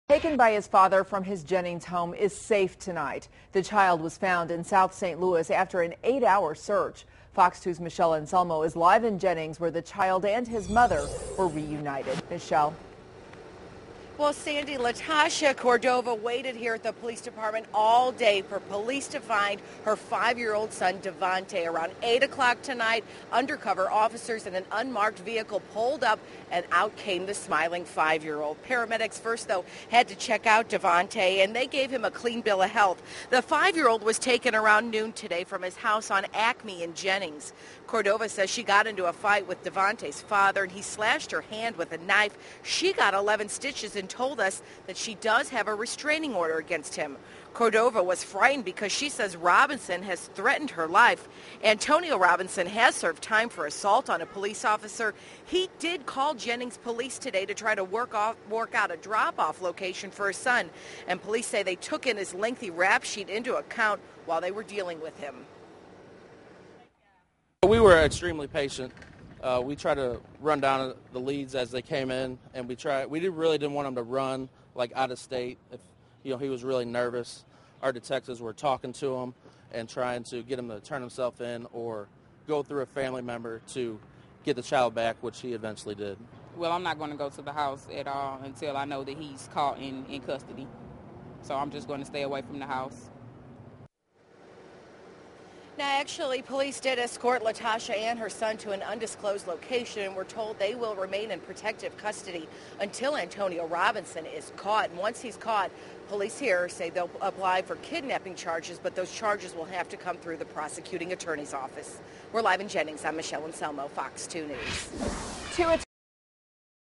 Missing Jennings Boy Found Safe
A boy taken by his father after fighting with his mother was found safe in south St. Louis.